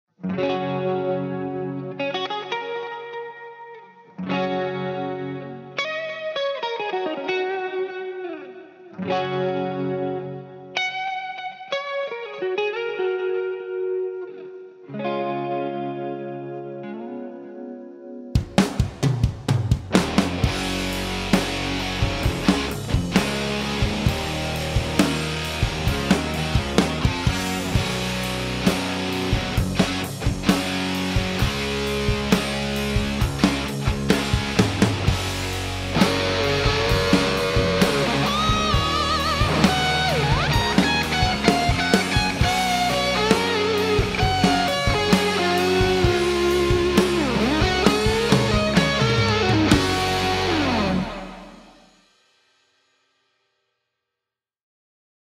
InstaJam #5: My New Guitar, the Gibson Les Paul GT
An original jam I wrote for a short Instagram video demonstrating my new Gibson Les Paul GT! I also played the drums and bass on this track.